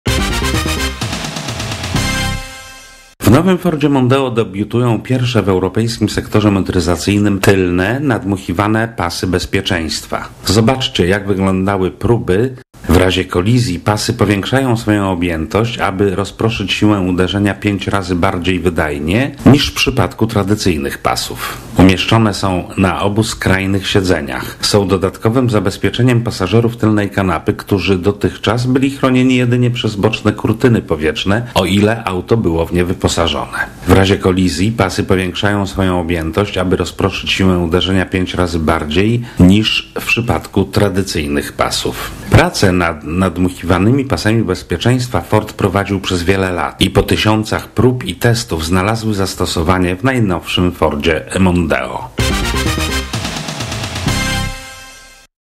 Jak to działa - Ford Mondeo - pasy z poduszkami powietrznymi
Innowacyjne pasy bezpieczeństwa w nowym Fordzie Mondeo. Na tylnych skrajnych siedzeniach zamontowano pasy, które są wyposażone w poduszki powietrzne.